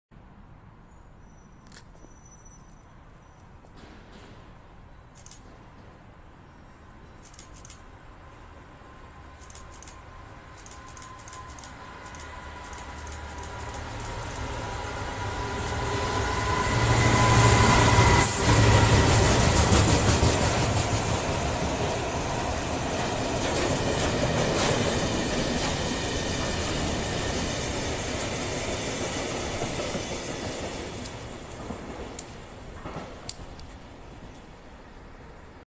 DB 66083 drags 555005 7Q42 Dollands Moor Sdgs to Wembley Receptions 1-7 @Ashford 29/03/23.
.Facebook Joe Roberts